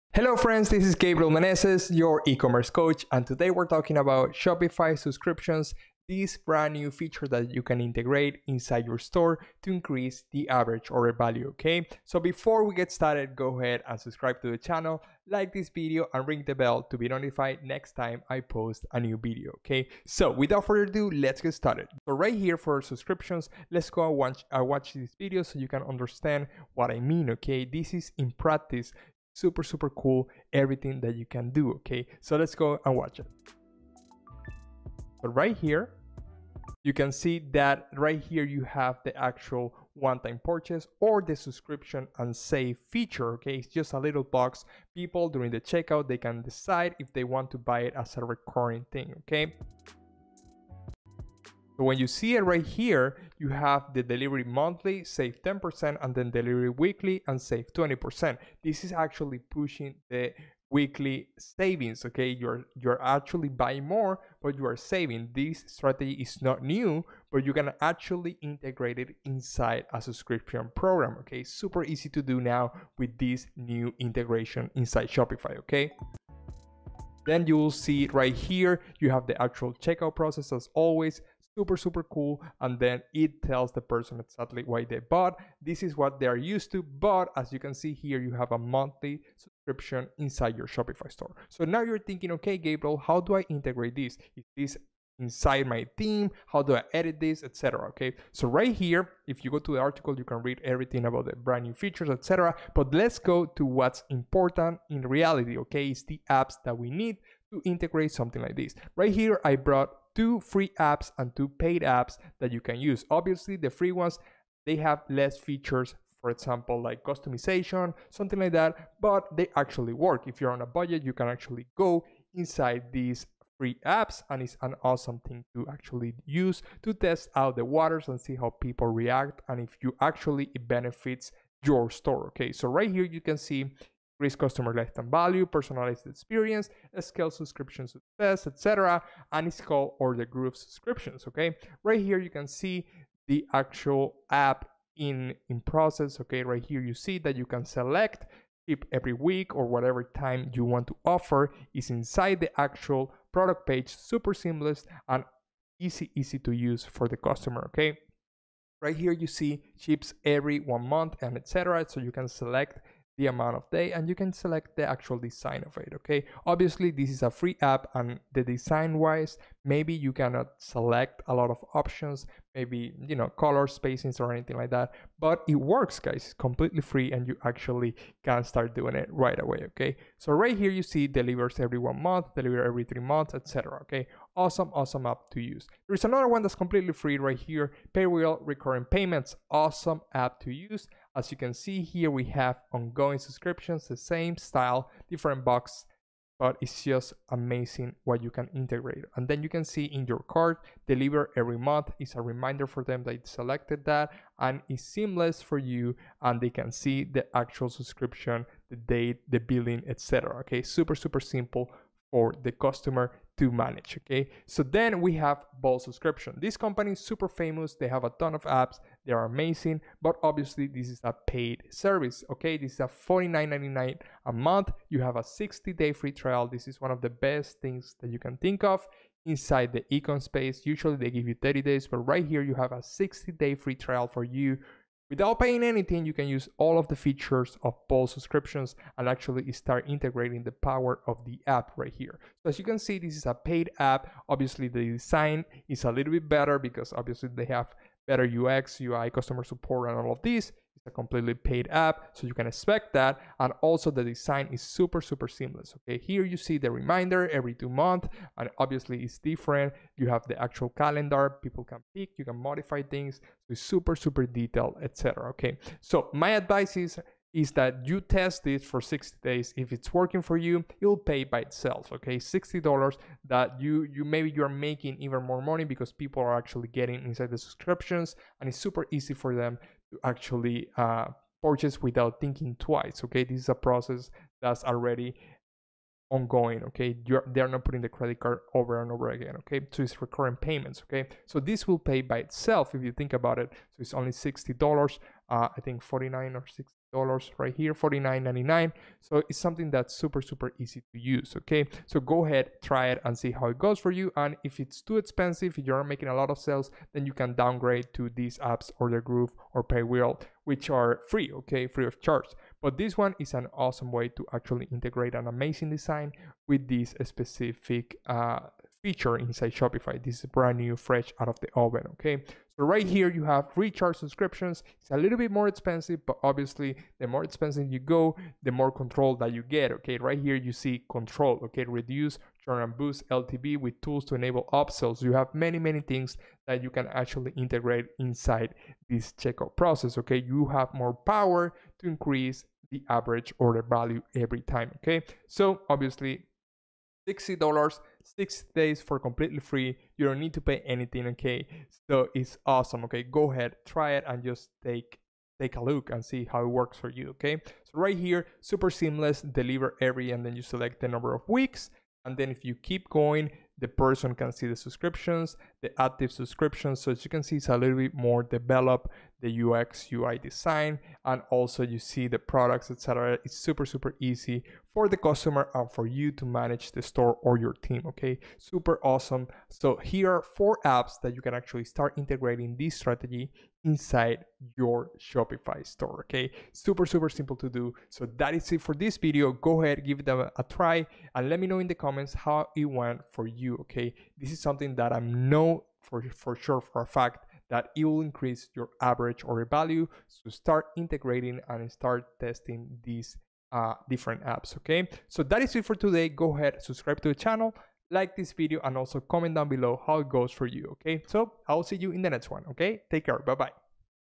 How to Set up a Recurring Payment in Shopify (4 Subscriptions Apps You Should Test)
👉 Free E-Commerce Tool Kit

👉 Favorite Tools